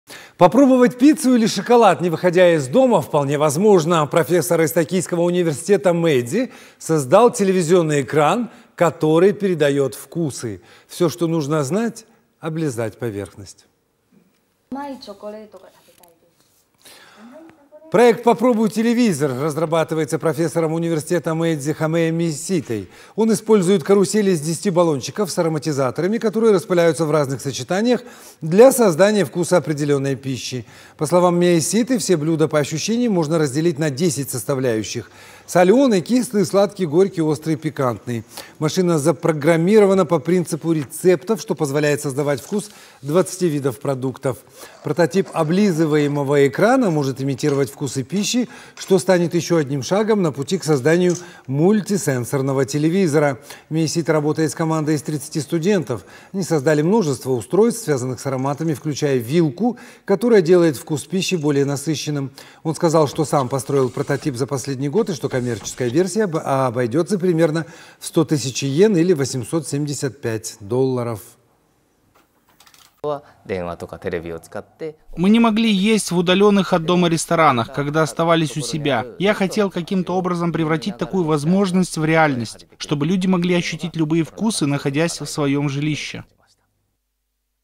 Телевизор, передающий вкусы, создали в Японии
Попробовать пиццу или шоколад, не выходя из дома вполне возможно. Профессор из токийского университета Мэйдзи создал телевизионный экран, который передает вкусы. Все, что нужно – облизать поверхность.
Проект "Попробуй телевизор" разрабатывается профессором университета Мэйдзи Хомеем Мияситой. Он использует карусель из 10 баллончиков с ароматизаторами, которые распыляются в разных сочетаниях для создания вкуса определенной пищи. По словам Мияситы, все блюда по ощущениям можно разделить на 10 составляющих, таких как соленый, кислый, сладкий, горький, острый и пикантный. Машина запрограммирована по принципу рецептов, что позволяет создавать вкус 20 видов продуктов. Прототип облизываемого экрана может имитировать вкусы пищи, что станет еще одним шагом на пути к созданию мультисенсорного телевизора. Миясита работает с командой из 30 студентов. Они создали множество устройств, связанных с ароматами, включая вилку, которая делает вкус пищи более насыщенным. Он сказал, что сам построил прототип за последний год, и что коммерческая версия обойдется примерно в сто тысяч иен или 875 долларов.
ХОМЕЙ МИЯСИТА, профессор университета Мэйдзи:
– Мы не могли есть в удаленных от дома ресторанах, когда оставались у себя. Я хотел каким-то образом превратить такую возможность в реальность, чтобы люди могли ощутить любые вкусы, находясь в своем жилище.